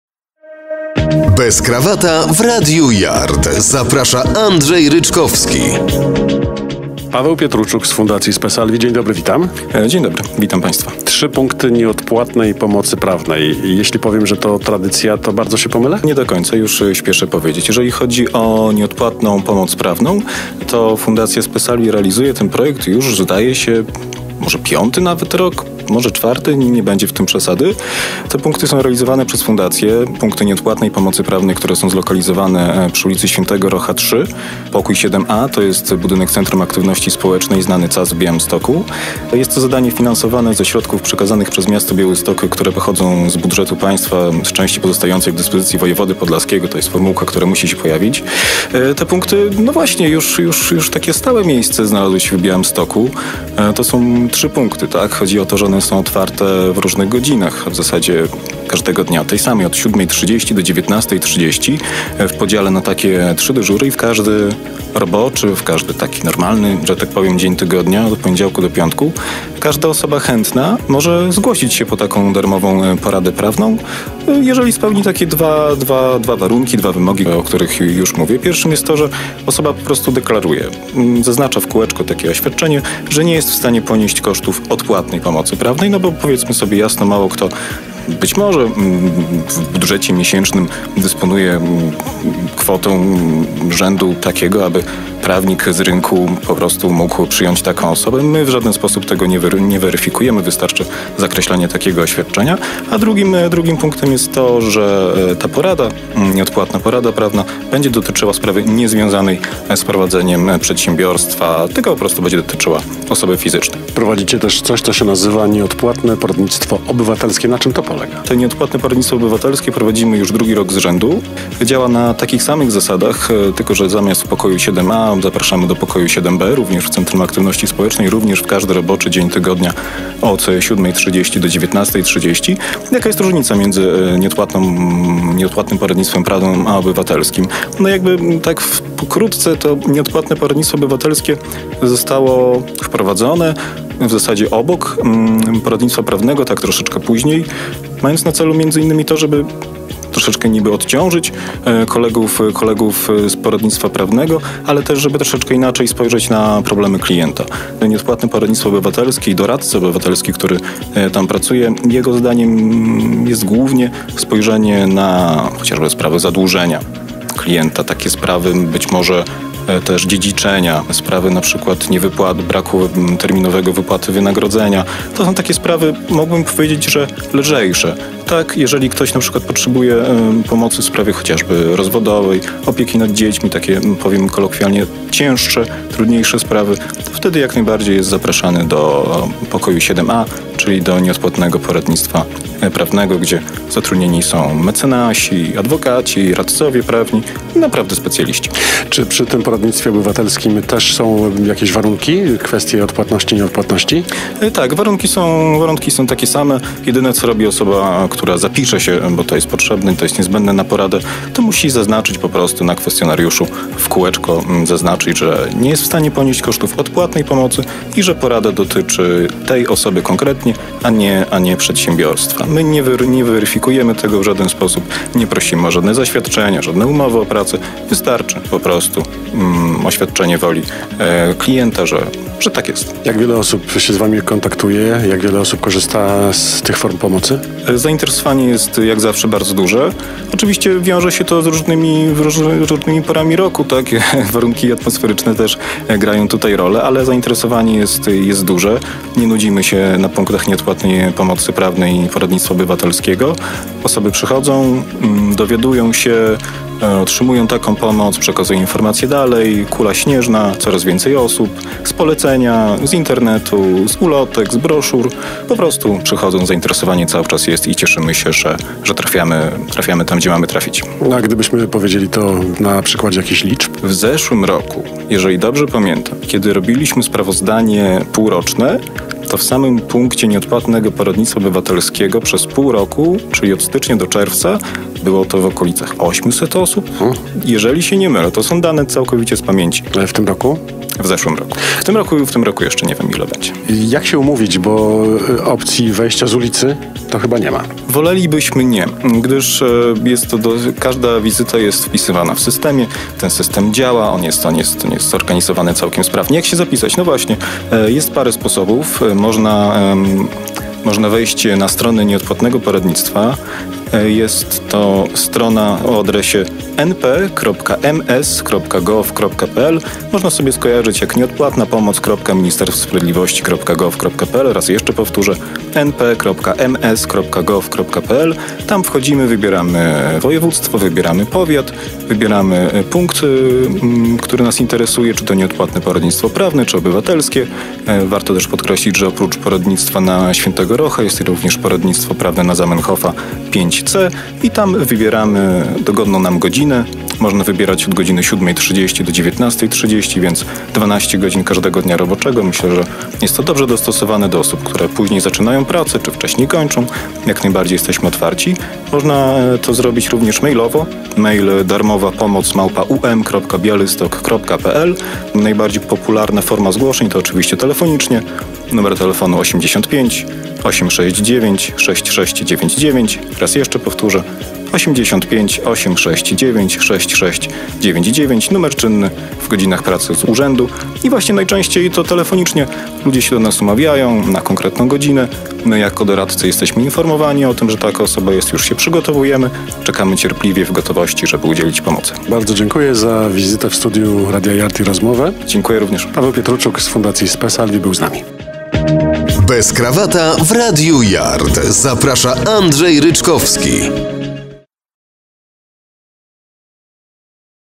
Paweł Pietruczuk
Fundacja Spe Salvi od kilku lat prowadzi punkty nieodpłatnej pomocy prawnej w Białymstoku. Dodatkowo, fundacja zajmuje się także nieodpłatnym poradnictwem obywatelskim.